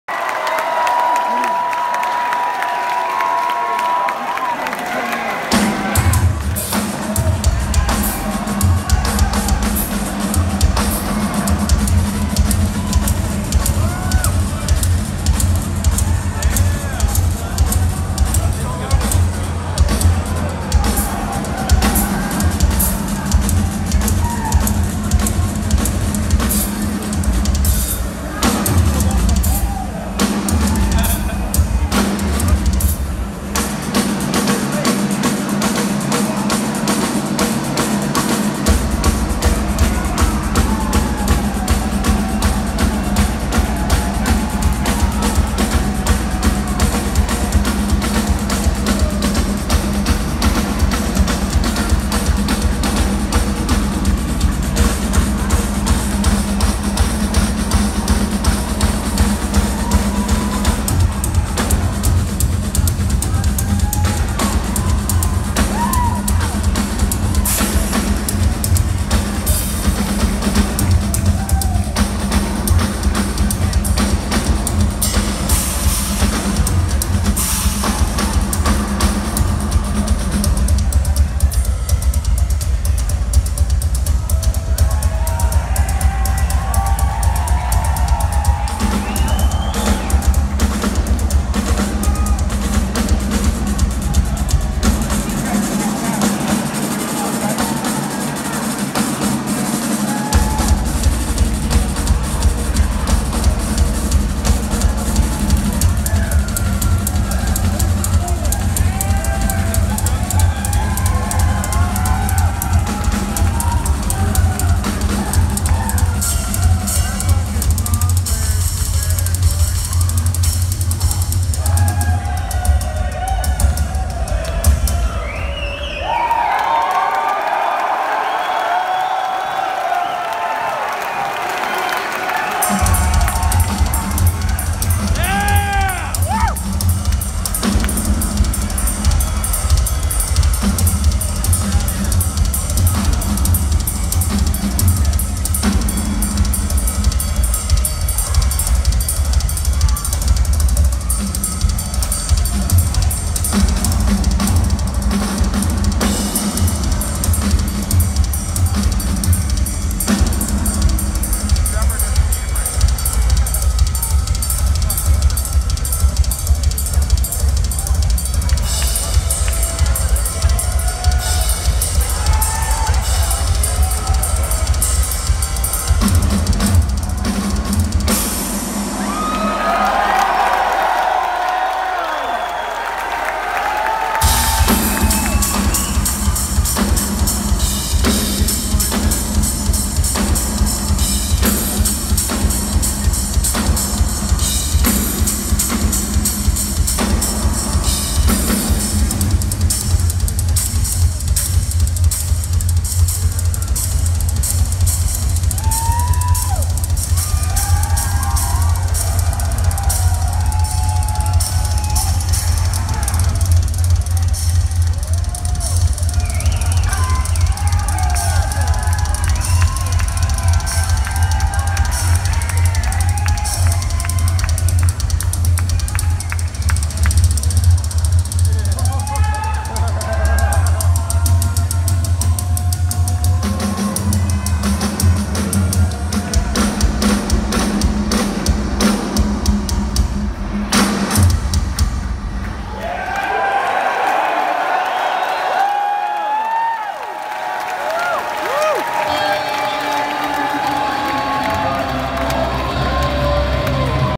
Gojira | Drum Solo Live | 8/02/2017
Subscribers of my channel probably know I created the Fallout 4 mod Gojira and World to Come. I named the beast after these guys. Finally got to hear them live and wow. This shit is nuts. The heaviest thing I have ever experienced.

Gojira!